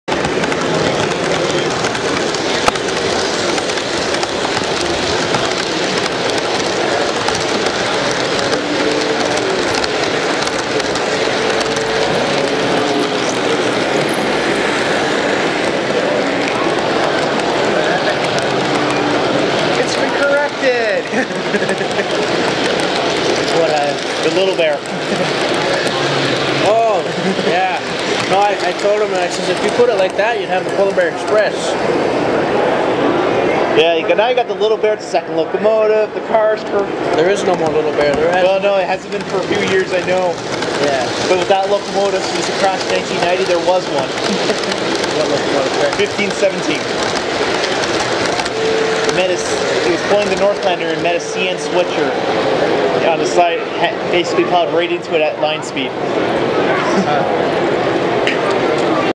Barrie Model Train Show
This is the Miner Junction Railway layout !
the owner of this layout needed to bleed his weasel, so he asked me to keep an eye on his layout while he was letting things flow !
NOTE: my new PROCOR tank car at the 0:25 !